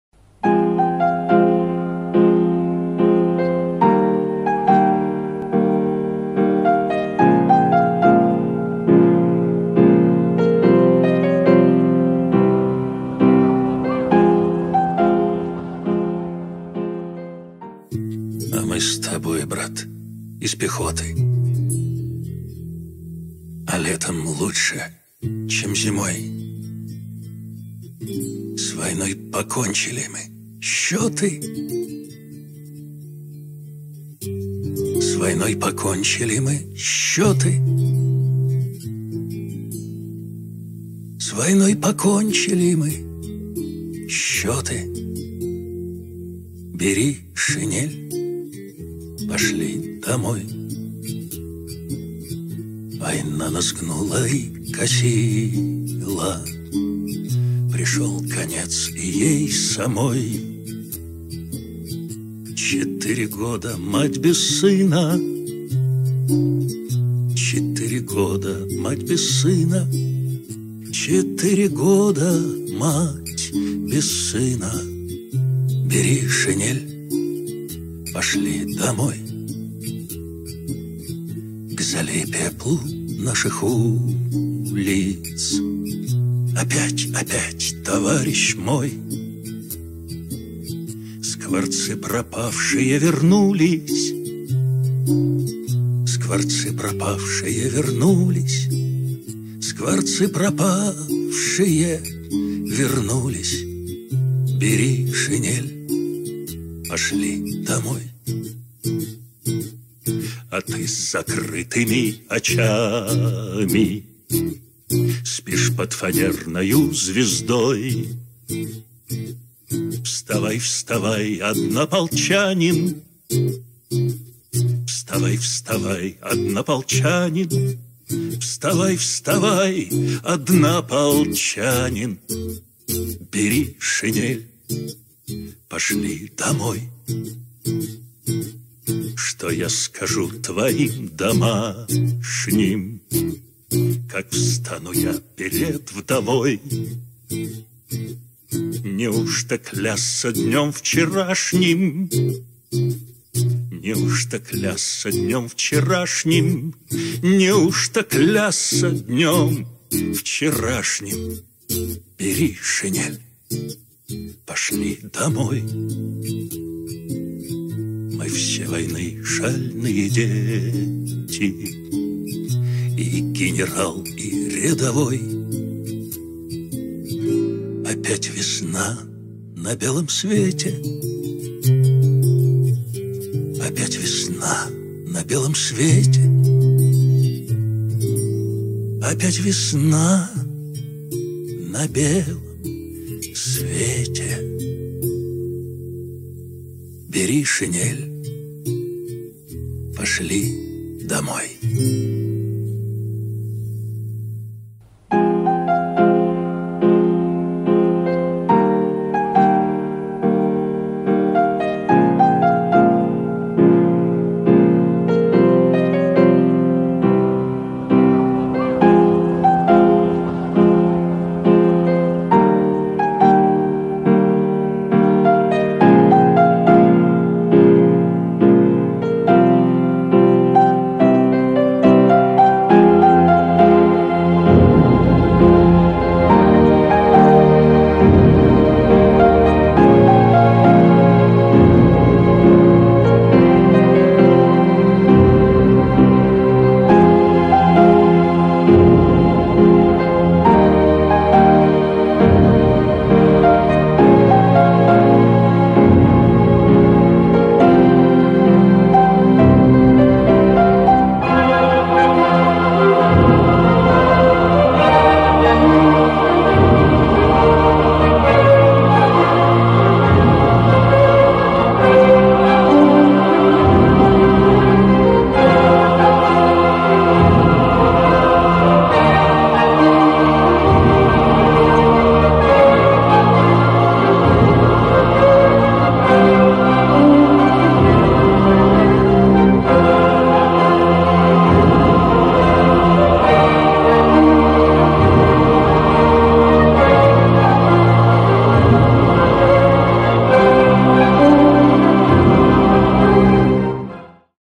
Бессмертный полк «Балтийского дома»
Актер Театра-фестиваля «Балтийский дом» Андрей Тенетко.
75-летию Победы